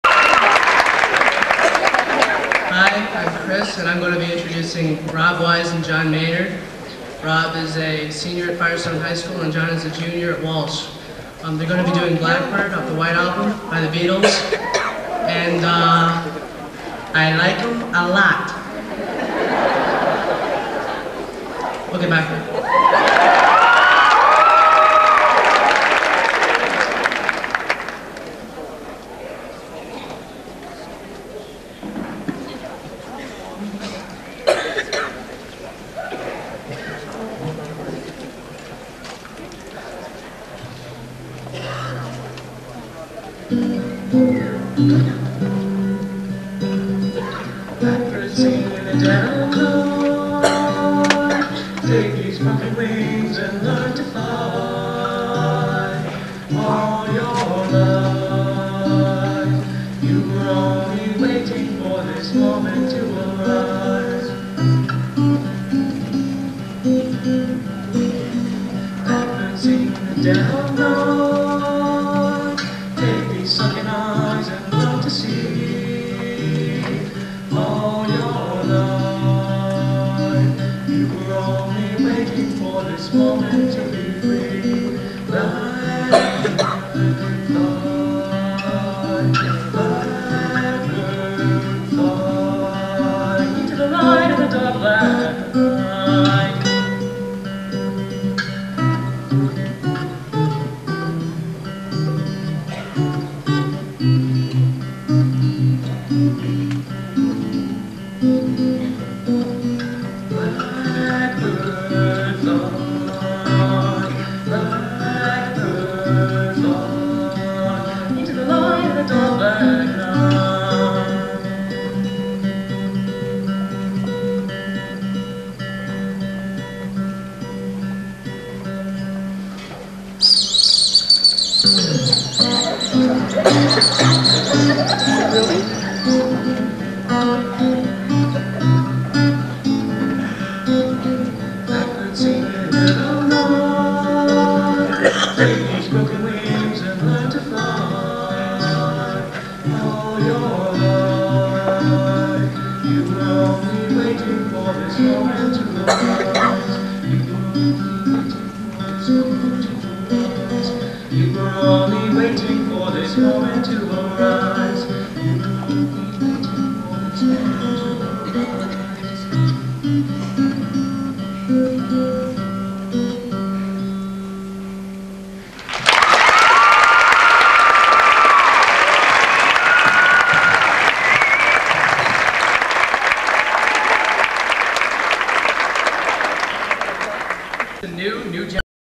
Rob WIse and Galen Maynard
Rob WIse and Galen Maynard - introduced by Chris Pierson  1995 Singing at the Summit